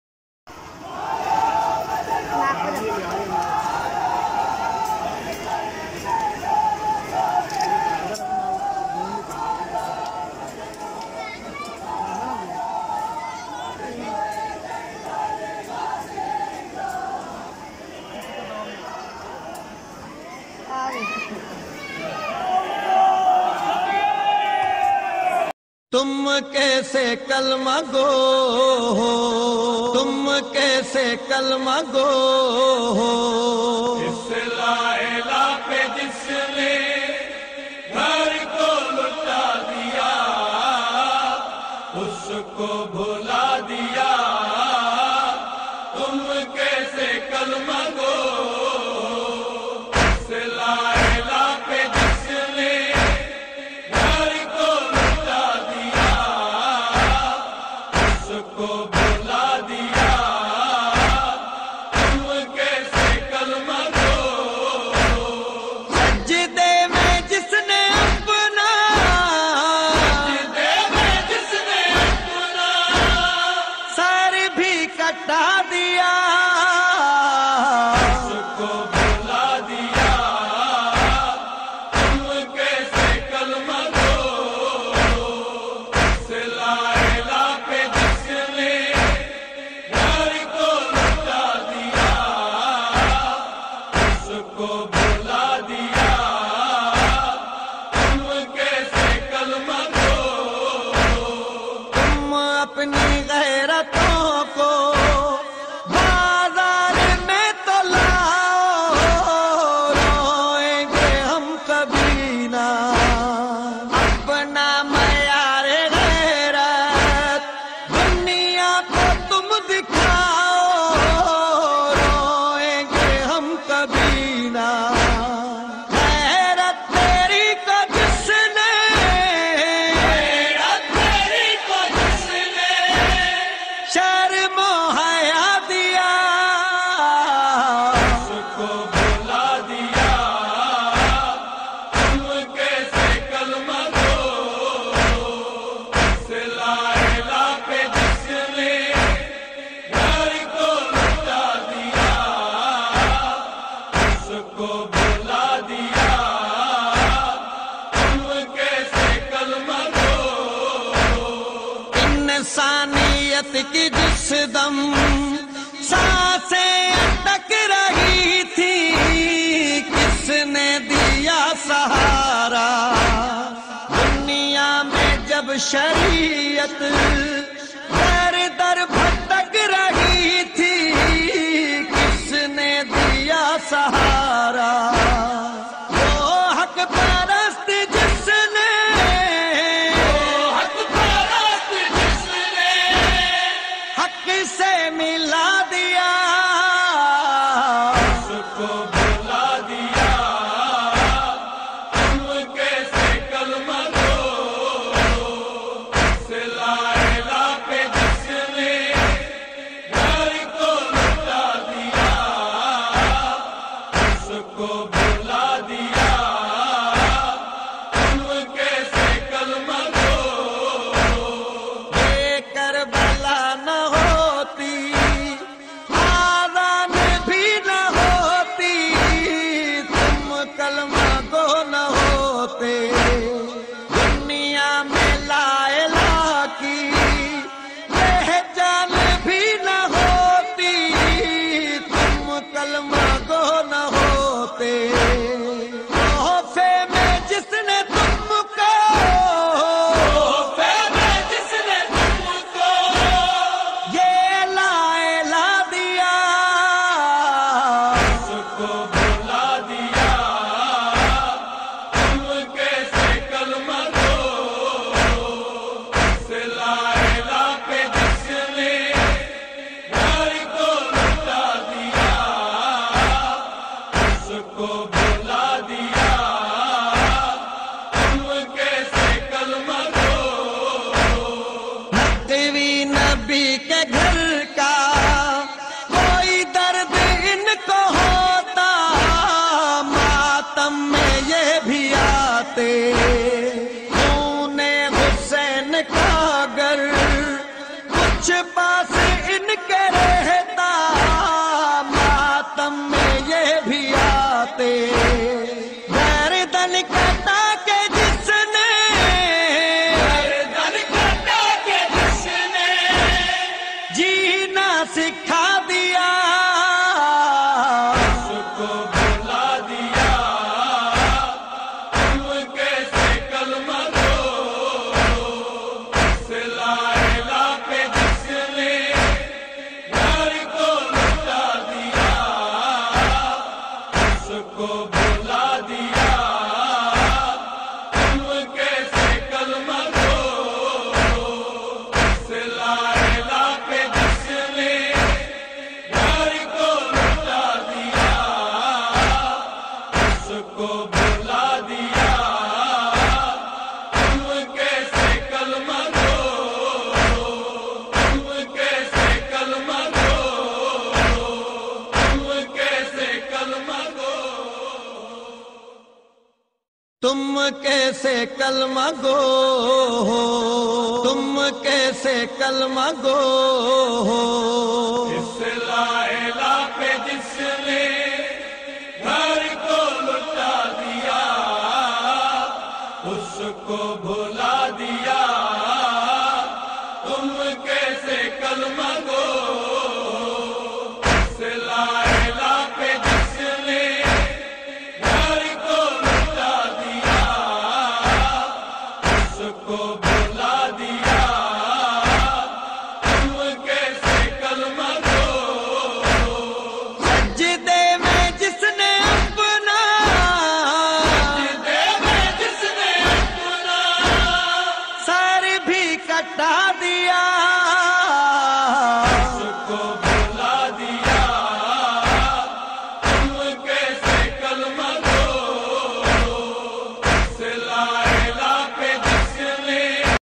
10 محرم الحرام 2019 امام بارگاہ دربار حسین علیہ السلام شیرگڑھ
مرکزی ماتمی دستہ فیض بندگی شیرگڑھ اوکاڑہ امام بارگاہ دربار حسین علیہ السلام محلہ شیخان والا شیرگڑھ